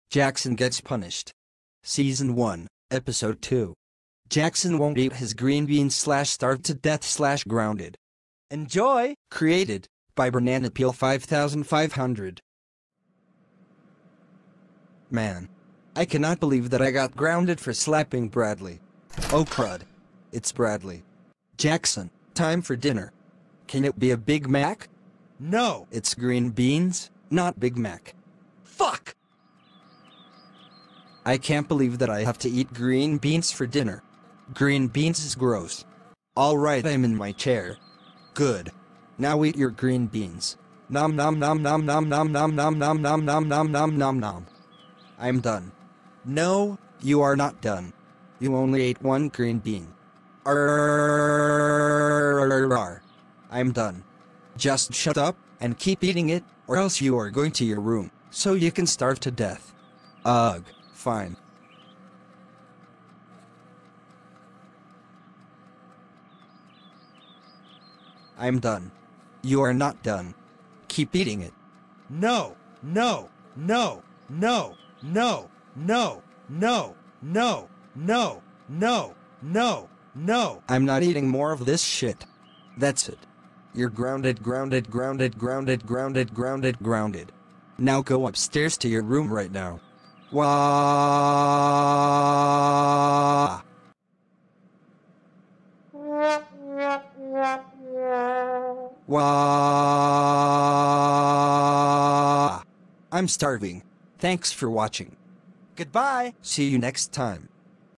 Jackson won't eat his green beans/starved to death/grounded
Jackson gets Punished
Season 1 Episode 2
Video created by, BananaPeel5500